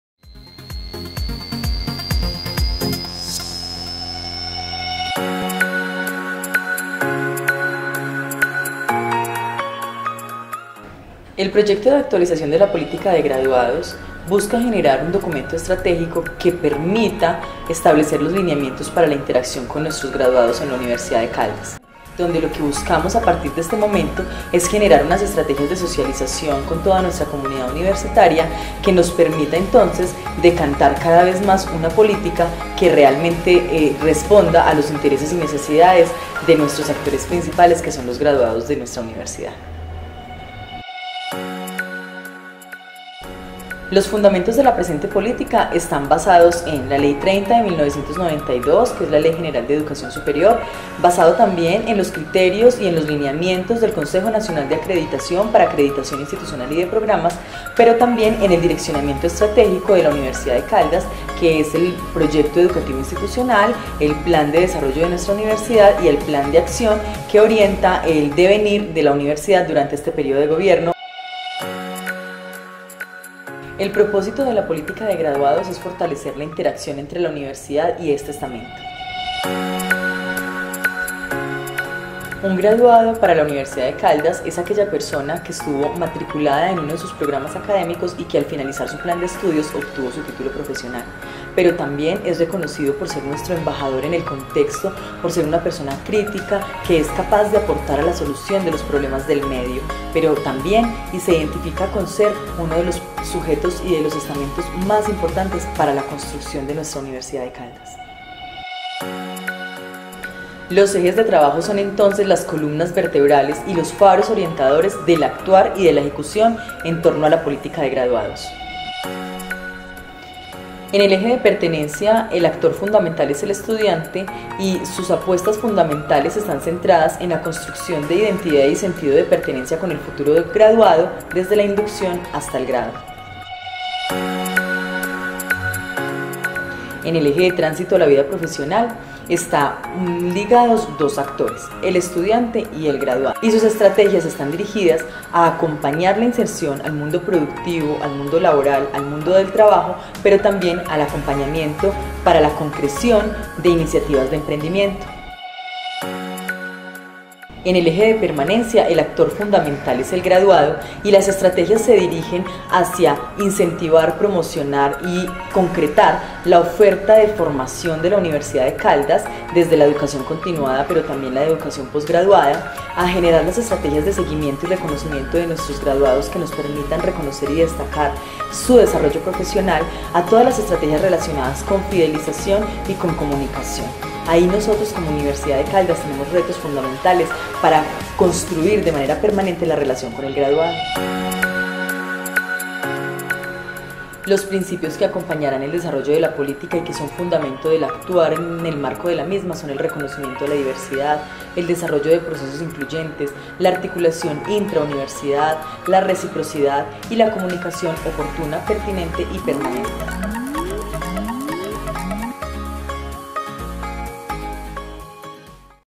Política de Graduados Universidad de Caldas 2017
Fortalecer la interacción con los graduados como una estrategia que favorece los procesos de calidad de la Educación Superior, en tanto los graduados sean reconocidos como embajadores institucionales que contribuyen desde su ámbito profesional, no sólo a la transformación social, política, económica, cultural y ambiental de diversos contextos de incidencia, sino además, a la  retroalimentación permanente de la formación universitaria.